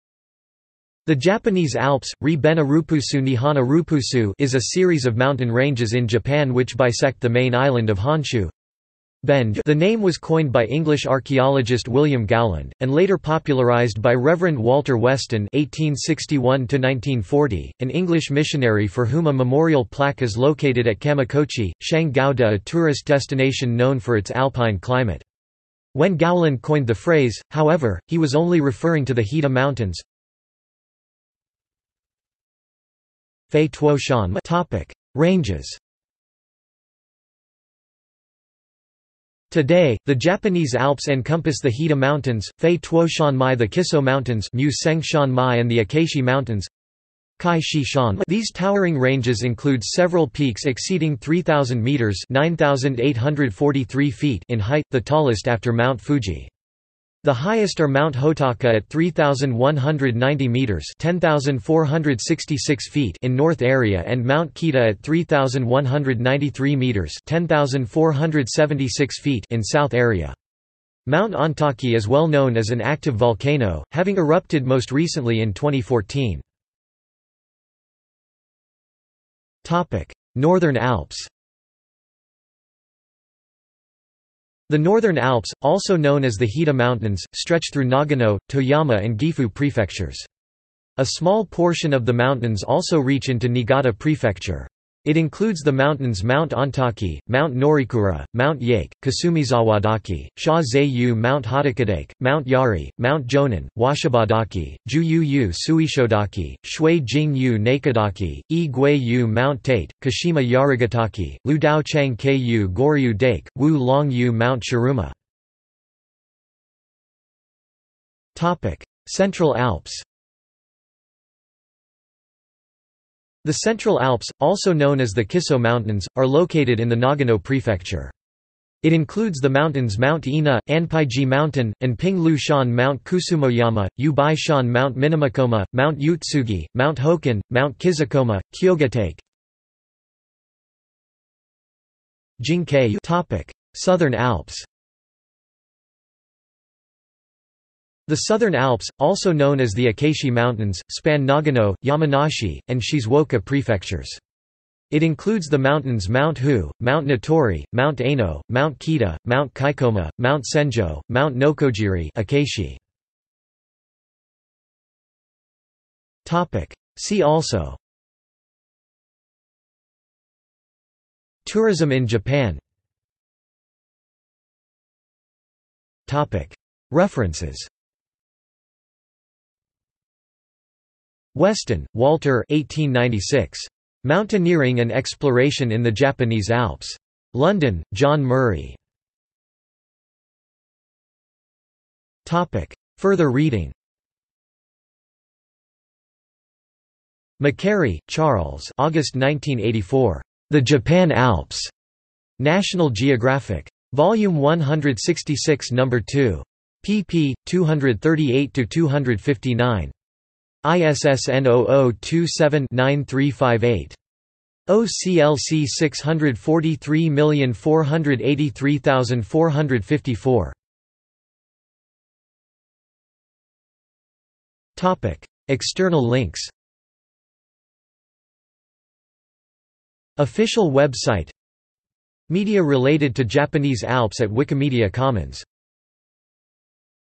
Japanese Alps | Wikipedia audio article
This is an audio version of the Wikipedia Article:
Japanese Alps

- Socrates

SUMMARY
The Japanese Alps (日本アルプス, Nihon Arupusu) is a series of mountain ranges in Japan which bisect the main island of Honshū (本州). The name was coined by English archaeologist William Gowland, and later popularized by Reverend Walter Weston (1861–1940), an English missionary for whom a memorial plaque is located at Kamikochi (上高地), a tourist destination known for its alpine climate. When Gowland coined the phrase, however, he was only referring to the Hida Mountains (飛騨山脈).